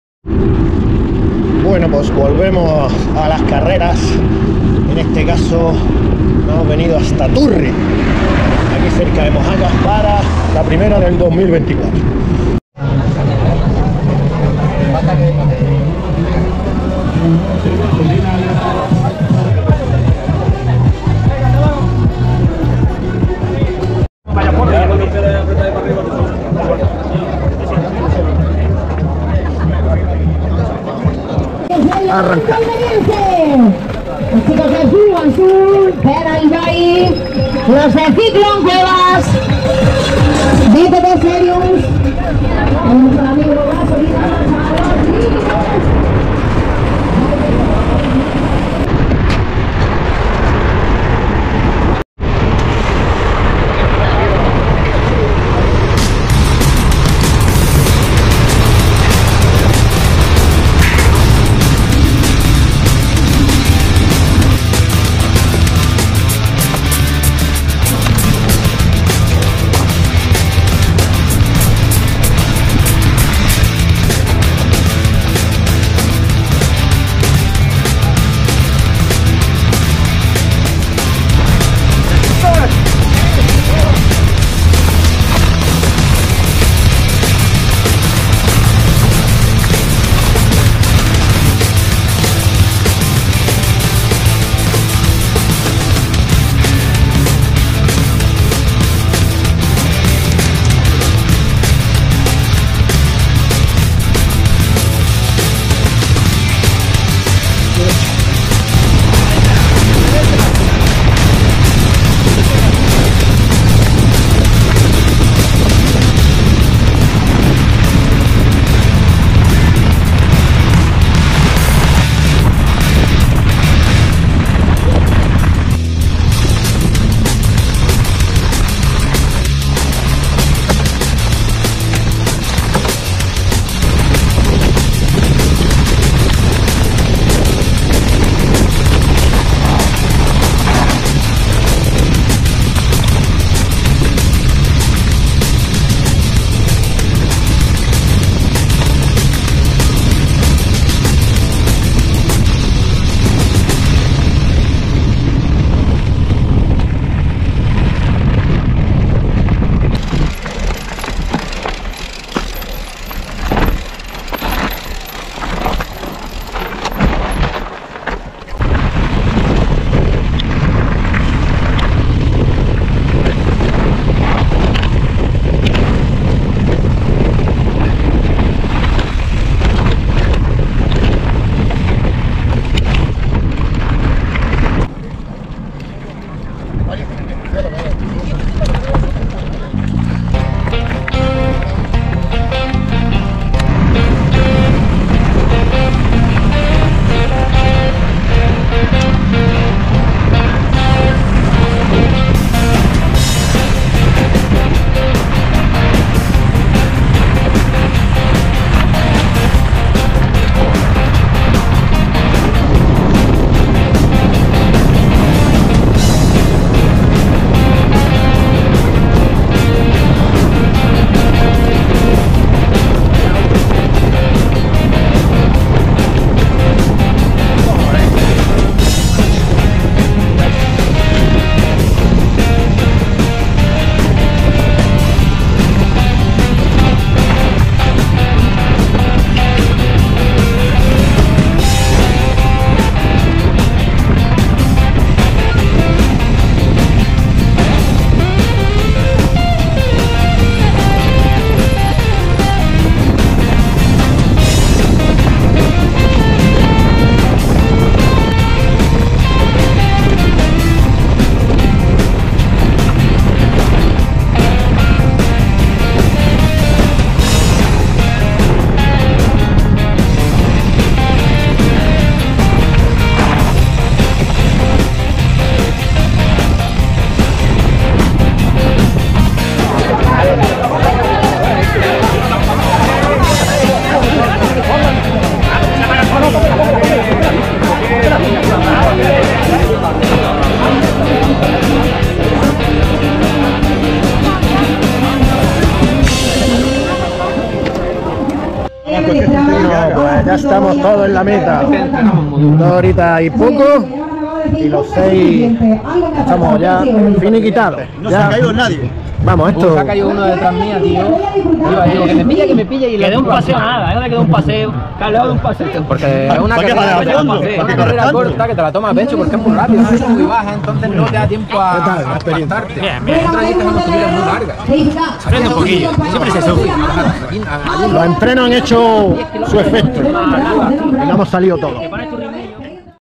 MTB Bomberos del Levante Almeriense, Turre 2024
Carrera de MTB disputada el 4 de Febrero 2024 en Turre (Almería) y organizada por los Bomberos del Levante Almeriense
37 kms , con algo más de 1000 metros de desnivel positivo. gran ambiente con casi 600 participantes 😜👌